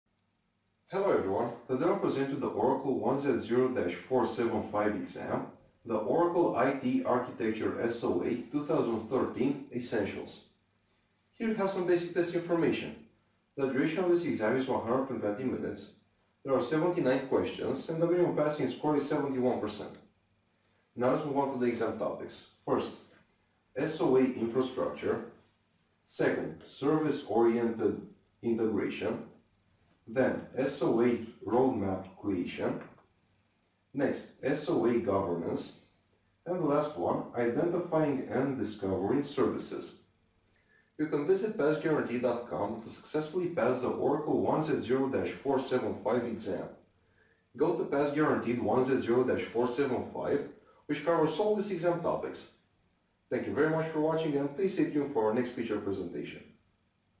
1Z0-475 – Oracle Exam IT Architecture SOA Test Essentials Questions
For more information on Oracle 1Z0-475 Practice Test Questions Please

The 1Z0-475 exam tests the candidate’s knowledge on foundation and expertise for Oracle Talent Management Cloud Service solutions. The 1Z0-475 exam also evaluates knowledge on the User Defined Fields topic.

Which are some of the topics of the 1Z0-475 Oracle SOA exam?

Test Topic 1: Service Oriented Architecture (SOA) Foundation Questions (Exam Coverage 20%)
Test Topic 2: Service-Oriented 1Z0-475 Test Integration Questions (Exam Coverage 20%)
Test Topic 3: Software Engineering Test in a 1Z0-475 SOA Environment Questions (Exam Coverage 25%)
Test Topic 4: Identifying and Discovering Test Services Questions (Exam Coverage 10%)
Test Topic 5: Determining 1Z0-475 ROI of SOA through Reuse Questions (Exam Coverage 25%)

Who can attend to the Oracle IT Architecture SOA 2013 Essentials test?

The Oracle SOA 1Z0-475 exam is designed for candidates able to better plan, execute, and manage your enterprise architecture and IT initiatives. The candidates must also possess knowledge on Project Selection Framework.

Can you give me some in-depth information on the 1Z0-475 exam topics?

• Describe the 1Z0-475 Deployment View of SOA infrastructure
• Describe the Process View of SOA infrastructure
• Describe the Development View of SOA infrastructure
• Describe the Development View of Service-Oriented Questions Integration
• Describe the Process View of Service-Oriented Integration
• Describe the Deployment View of Service-Oriented Questions Integration
• Describe the process for creating a roadmap for a strategic SOA initiative
• Describe the key concepts for the 1Z0-475 SOA Maturity Model

What’s the 1Z0-475 passing score and duration?

The duration of this exam is 120 minutes (79 questions) and the minimum passing score is 71%.